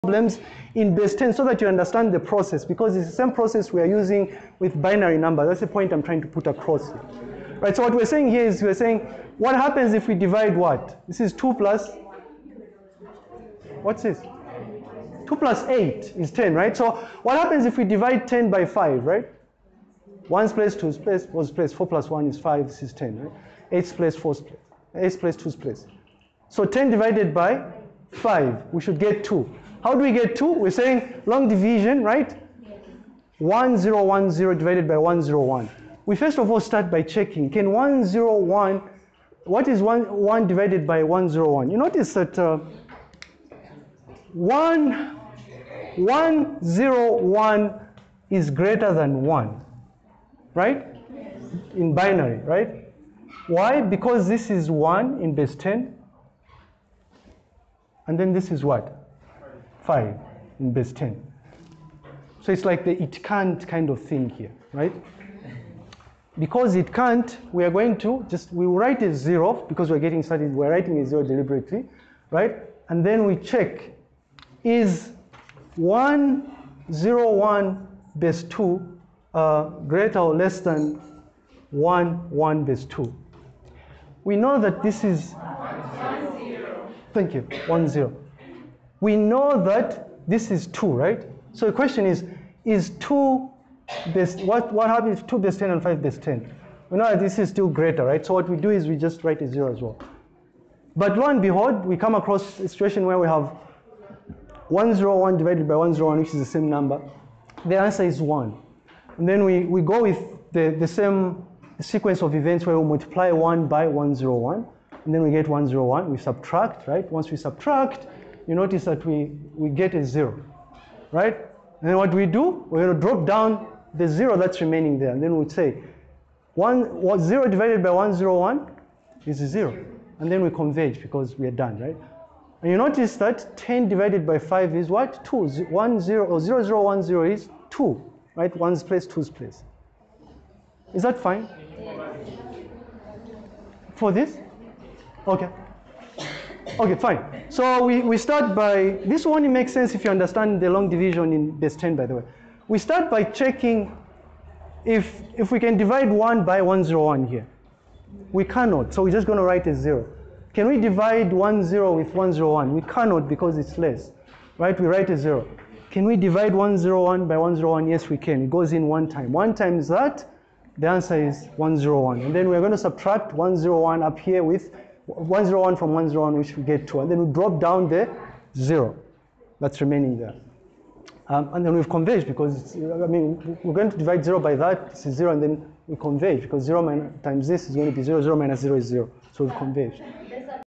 [Illustration] Number Systems and Representation: Binary Division #1
Excerpt from Lecture Series 17 No. 2: Number Systems and Representation [1, 2, 3].

In class live illustration of binary division